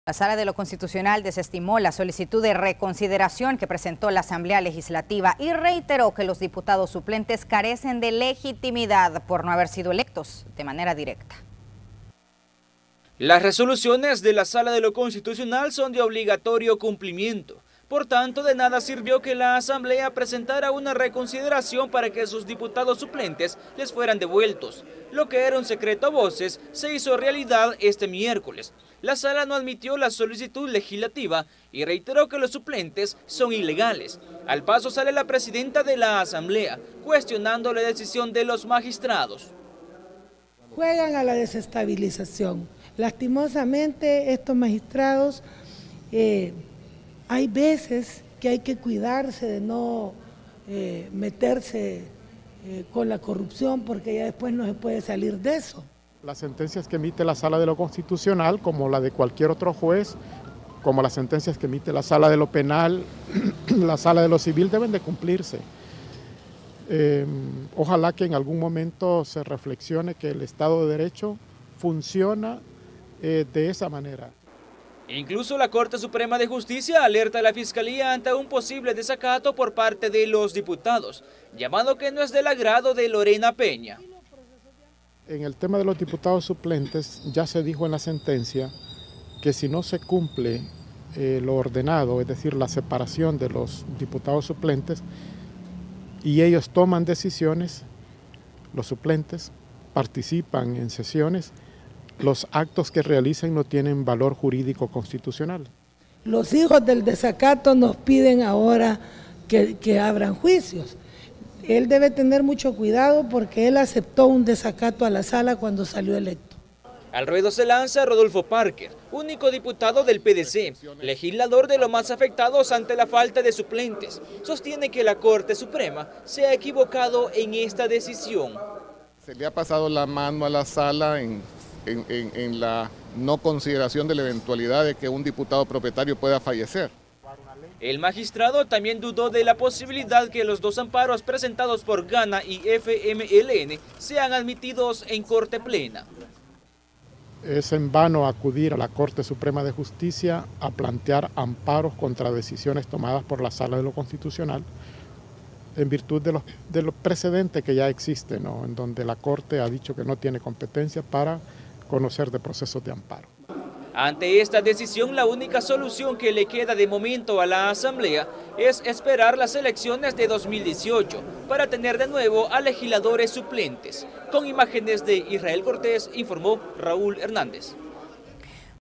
Rechazan recurso para revertir fallo sobre ilegitimidad de diputados suplentes
Acción interpuestas por la Asamblea Legislativa no fue admitida por la Sala de Lo Constitucional. Lorena Peña criticó resolución